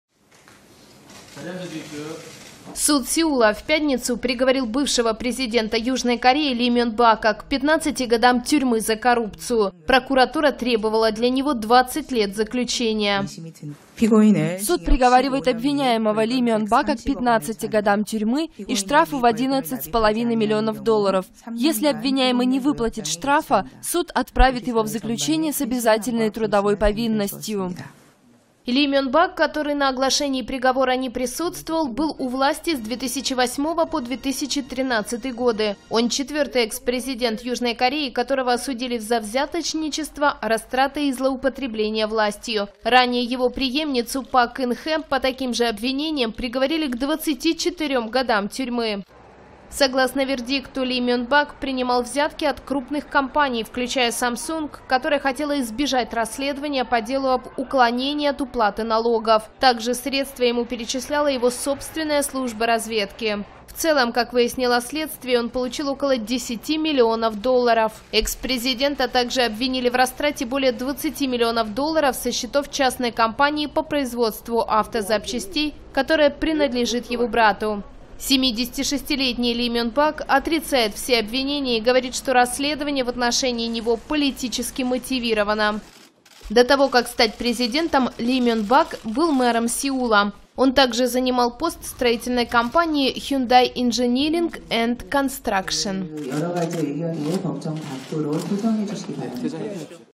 Экс-президента Ли Мён Бака посадили на 15 лет за коррупцию
Бывшего президента Южной Кореи приговорили к 15 годам тюрьмы. Его обвинили в коррупции и взяточничестве.